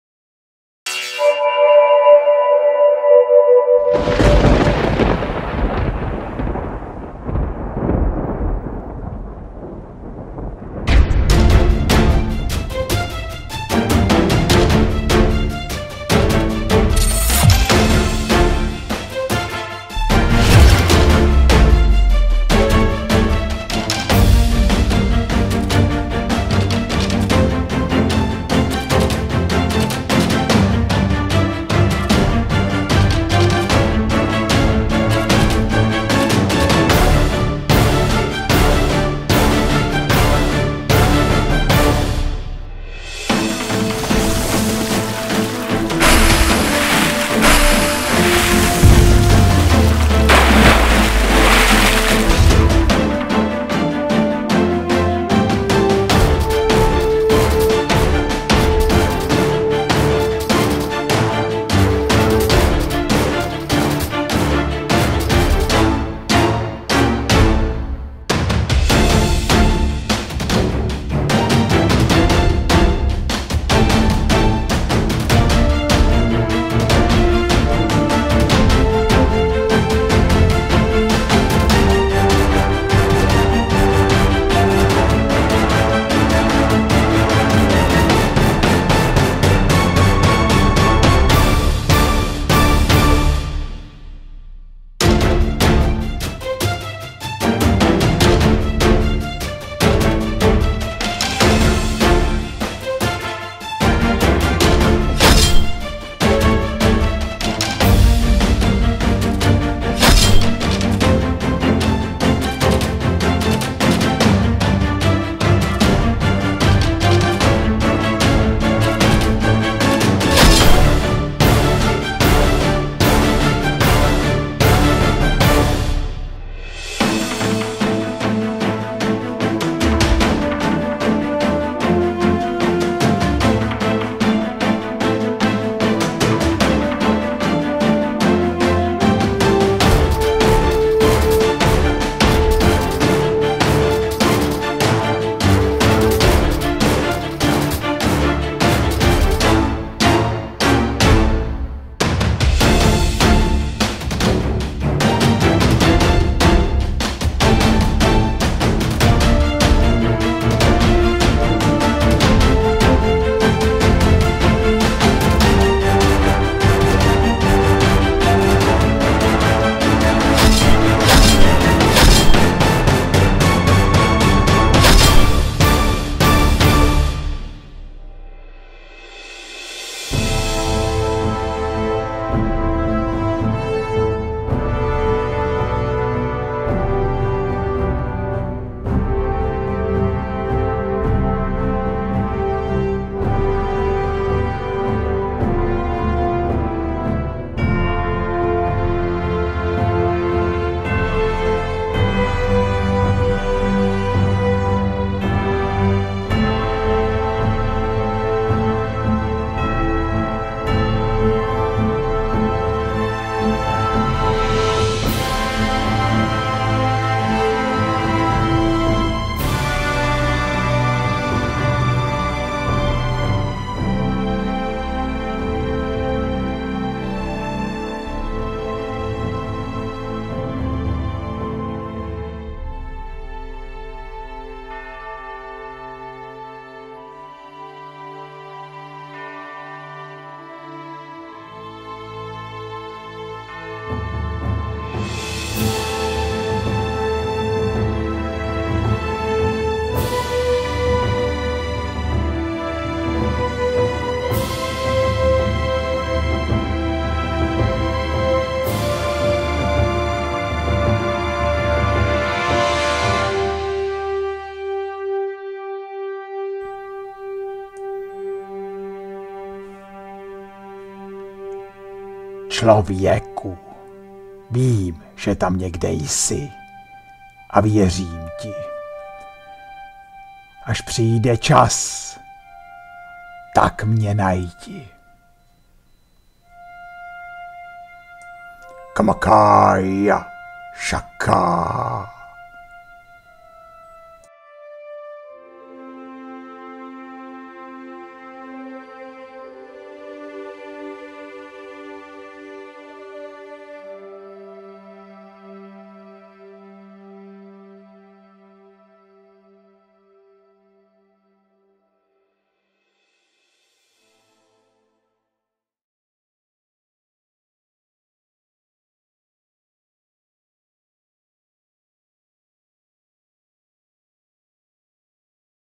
Fantasy tábory - CK Beruška
Fantasy tábory pro dobrodruhy. Je čas na velkou výpravu do světa magie, který je více jak skutečný.